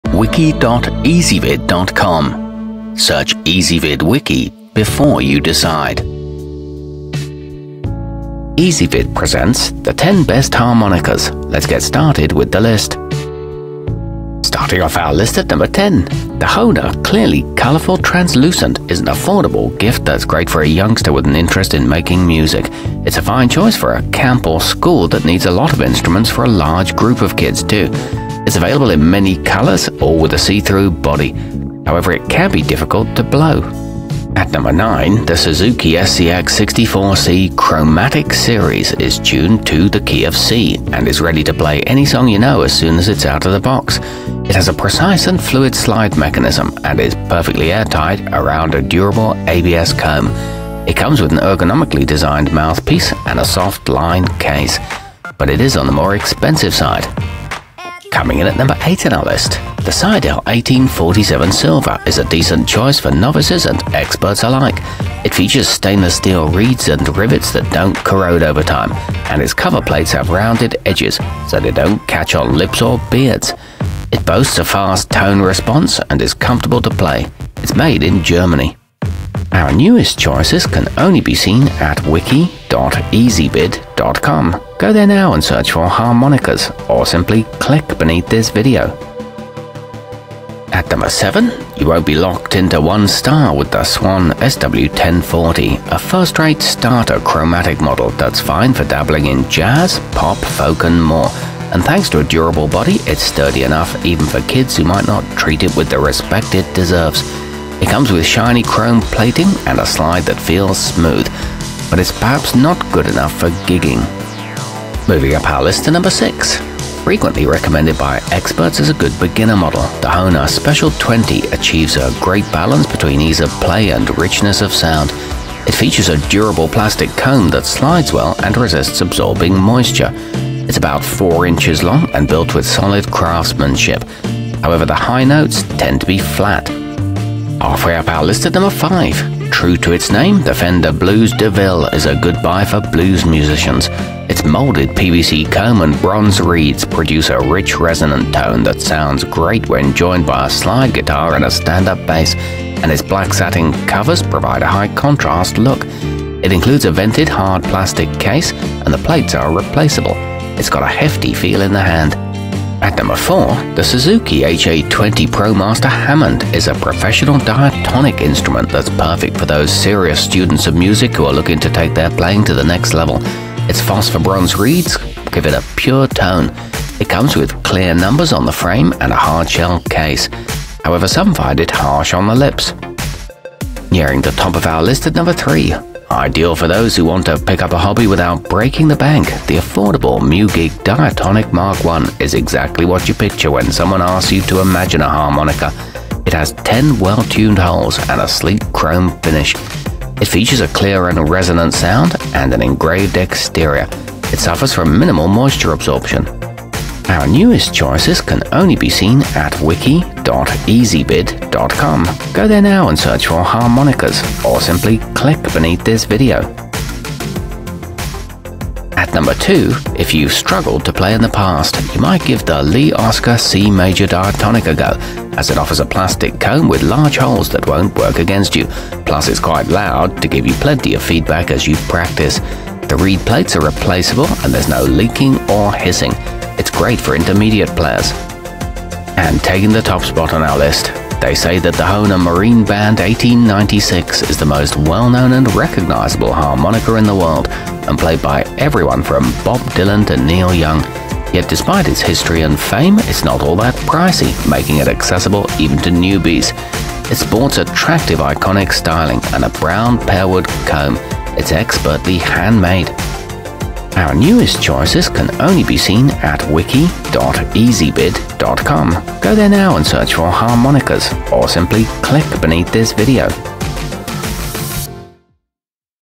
10 Best Harmonicas 2019
Our complete review, including our selection for the year's best harmonica, is exclusively available on Ezvid Wiki.

Harmonicas included in this wiki include the mugig diatonic mk-1, hohner marine band 1896, suzuki scx-64c chromatix series, hohner special 20, fender blues deville, swan sw1040, hohner clearly colorful translucent, seydel 1847 silver, suzuki ha-20 promaster hammond, and lee oskar c major diatonic.